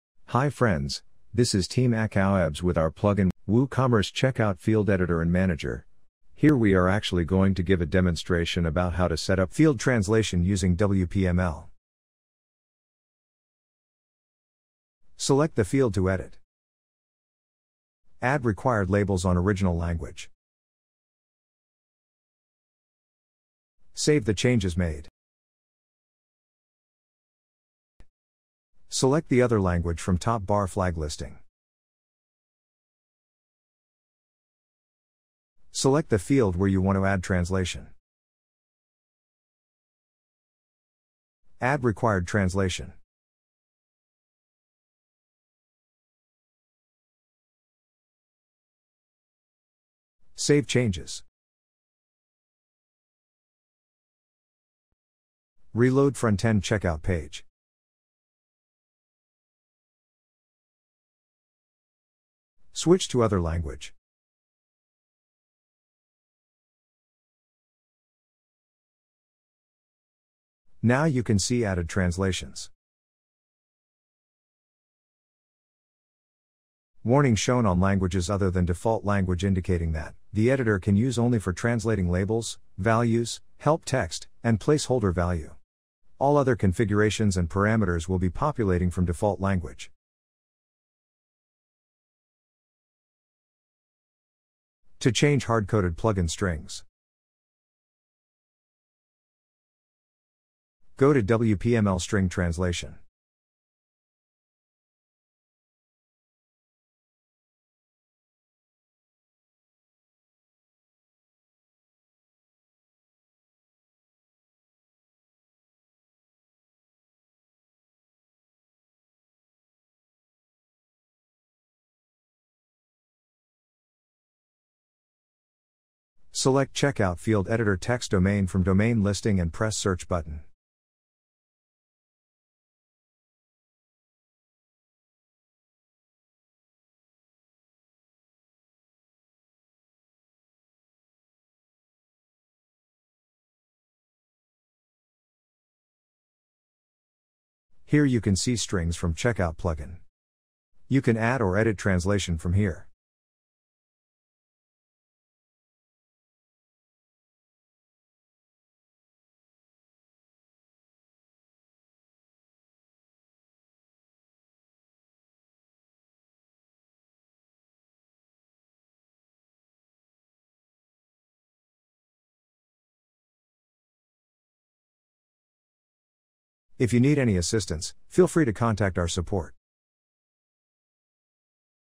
Field Translation WPML | Woocommerce Checkout Feild Editor & Manager | wordpress Plugins
Woocommerce Checkout Feild Editor & Manager

Features-
-20+ types of custom Checkout Fields
-Custom sections Add New Sections to Almost 13 Places on the checkout page
-Conditional Logic – Show or hide some fields & sections based on the value selected for other fields
-Fields & sections based on Products, categories, user role & cart total - Show or hide some fields & sections based on the products & categories added to the cart, user role, cart subtotal & cart total.
-Show Fields in Order Details and Email - manage field display on the order email and order details page.
-WPML and PolyLang Support